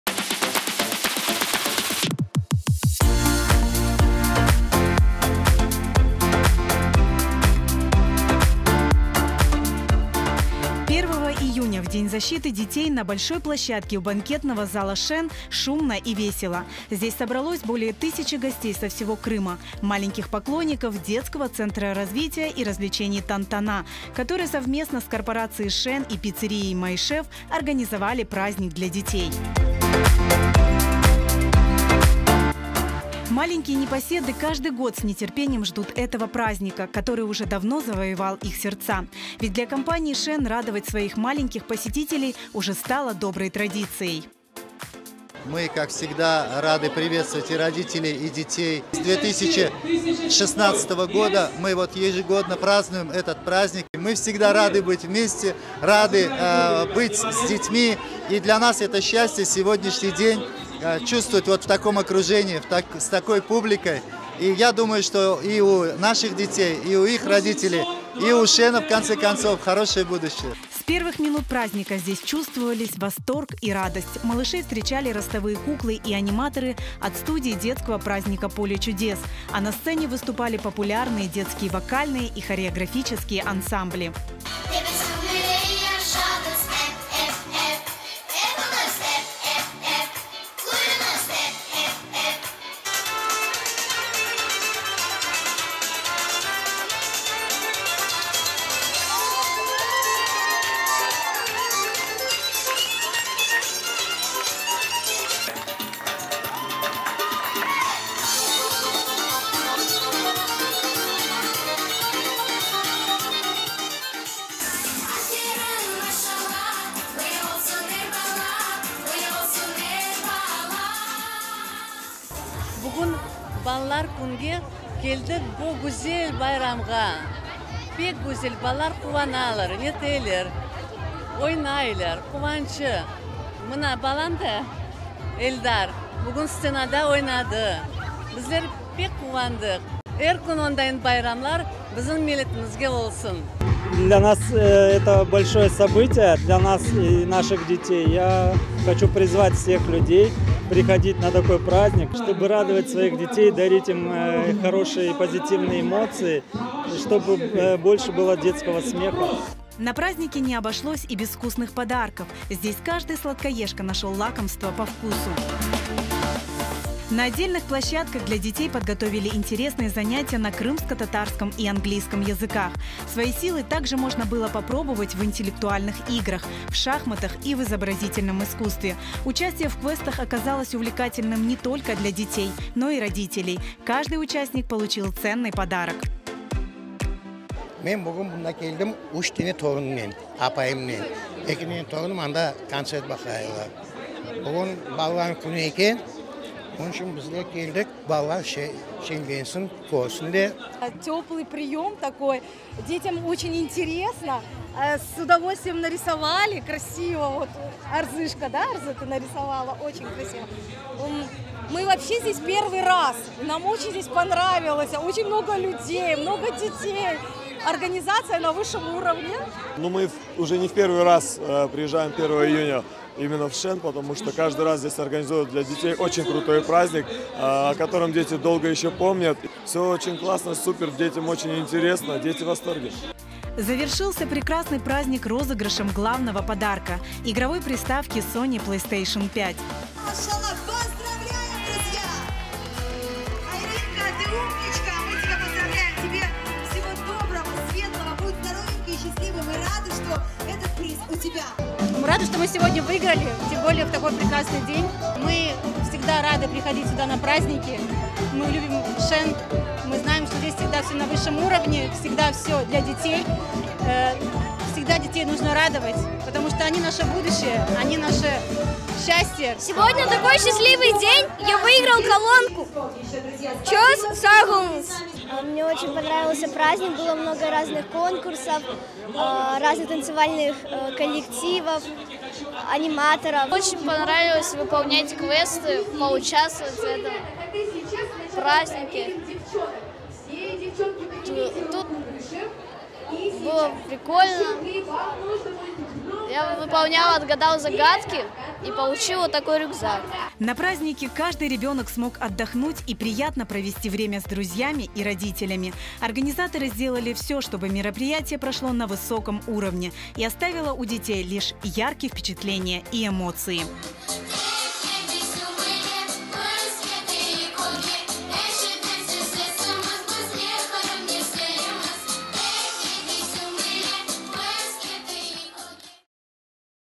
"Shen" kompaniyaları gruppasından balalar içün bayram
"Shen" kompaniyaları gruppası  Balalarnı qorçalav künü şerefine kenceler  içün qocaman bir bayram teşkil etti. 1200-den ziyade kiçik musafirlerni tatlılar, dondurma ve bahşışlar ile qarşıladılar.  Balalar içün meraqlı meşğuliyetler, "Pole çudes" bala bayram studiyasınıñ animatorları ile eglenceler keçirildi. 10 bala muzıka ve oyun taqımlarınıñ çıqışları ise aqiqiy bayram müitini yarattılar.